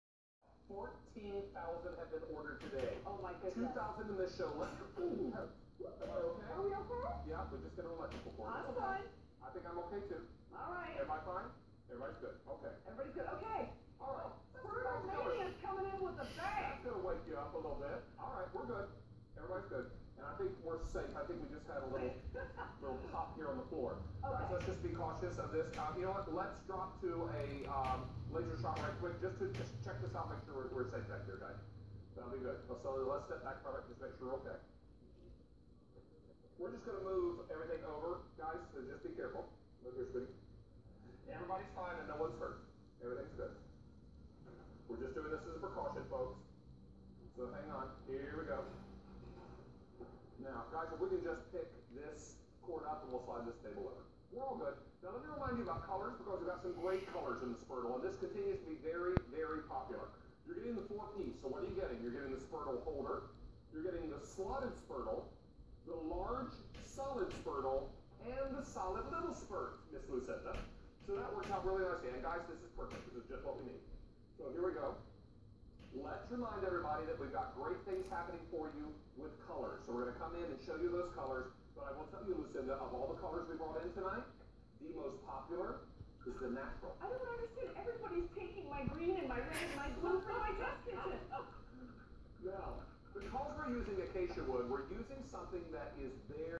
QVC Shocker
Electrical spark during live show. Great recovery by personalities.